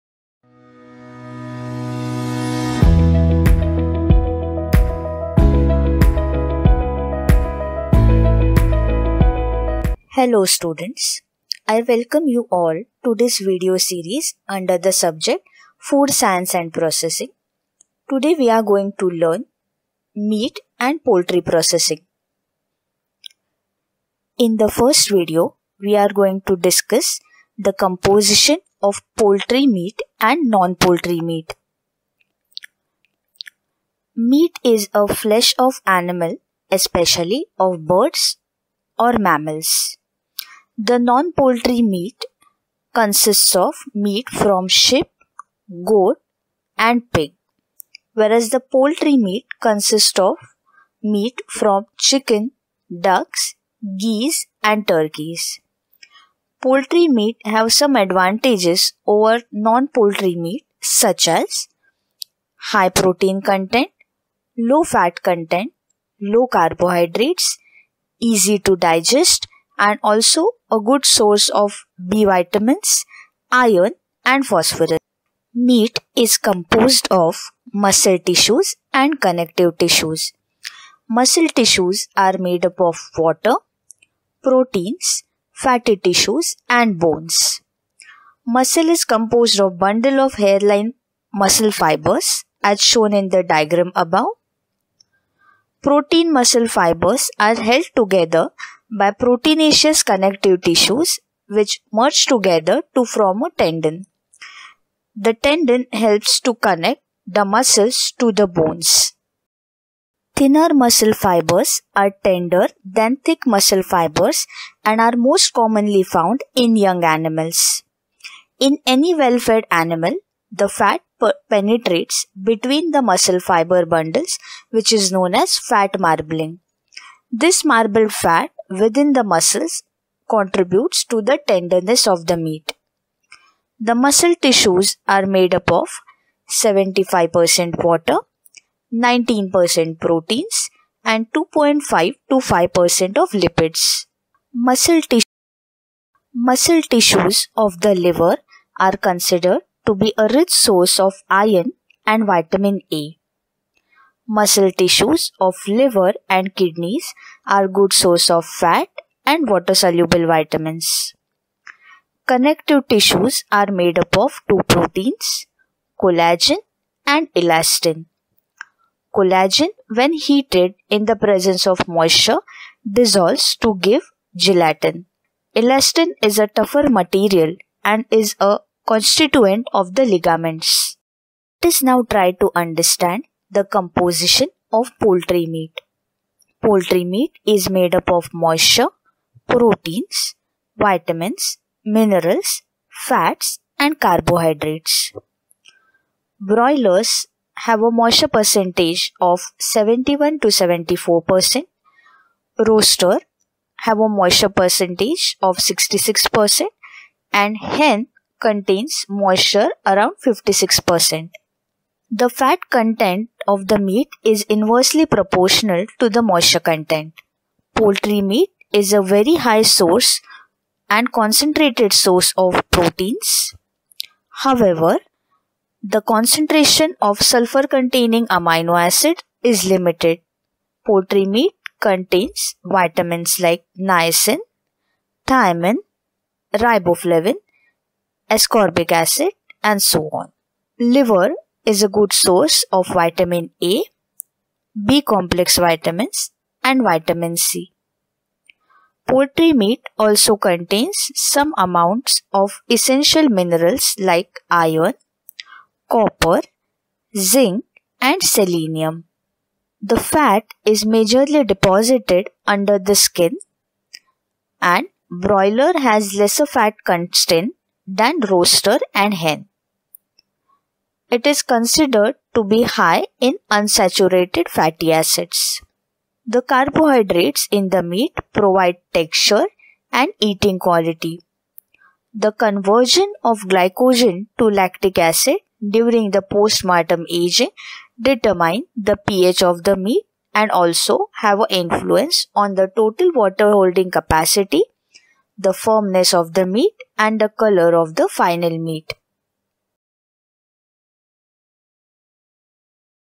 Meat and Poultry- Food Science and Processing
by Ms. Madhura S. Ghaisas, Department of Food Science and Nutrition
Semester V
T.Y.F.S.N.